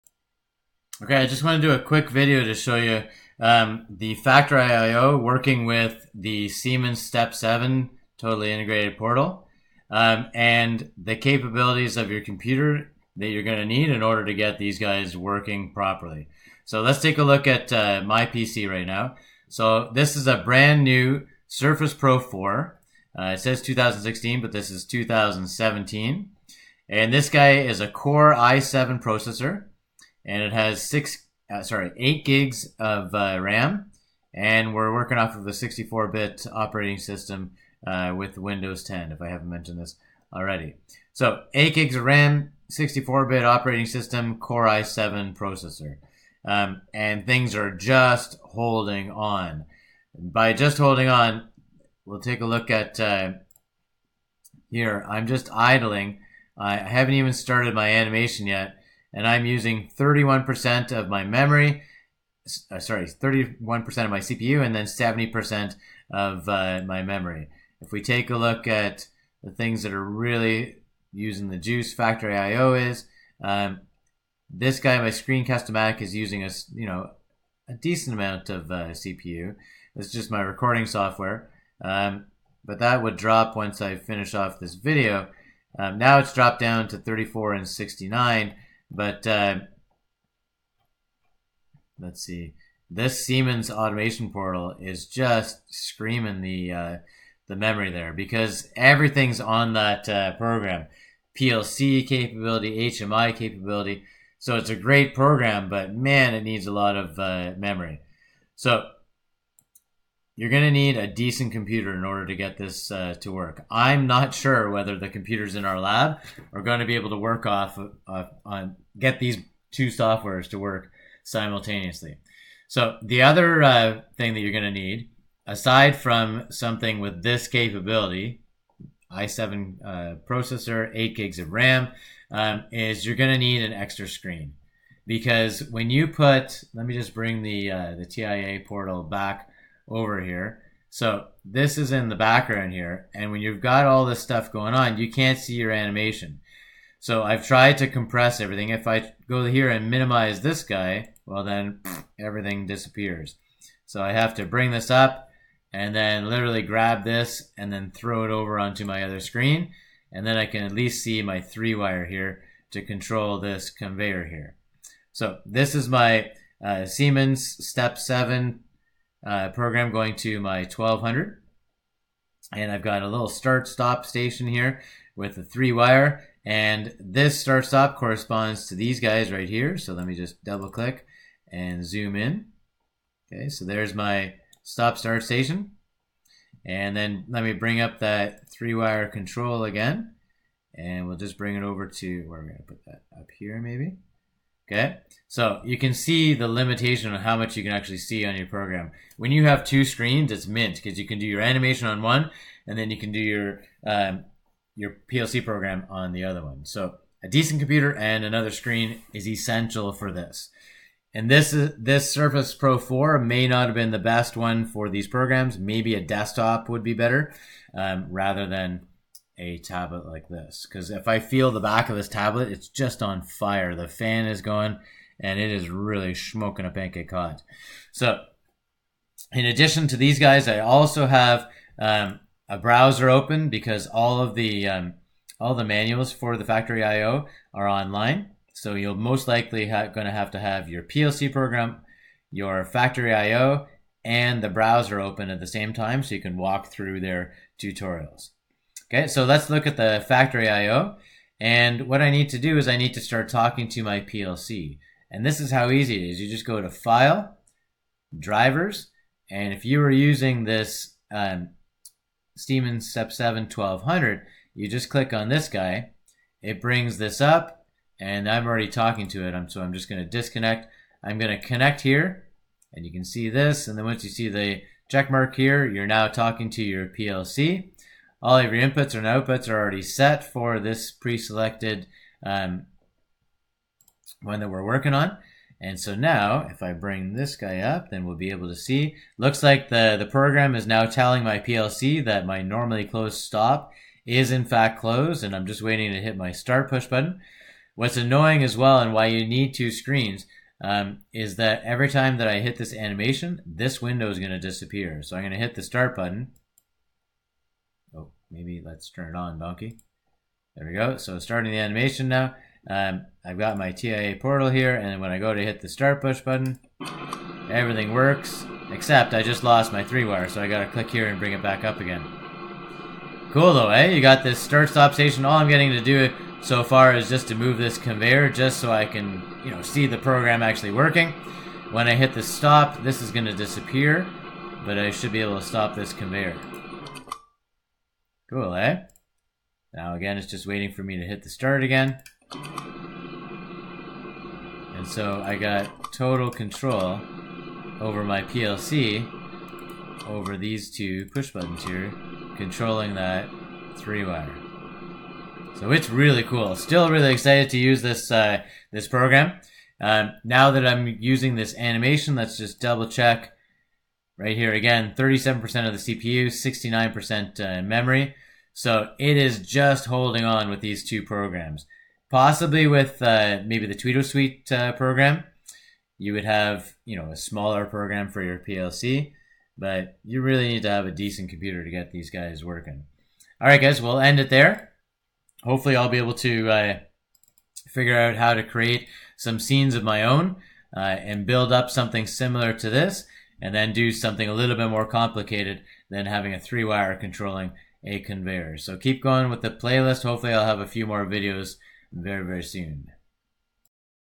Factory IO Tutorial #2 (What computer specs do I need to control Factory IO with Siemens TIA Portal)
Pretty sure you're going to need a Core i7 Processor, 8GB of RAM and a Second Screen to get Factory IO and Siemens TIA Portal working seamlessly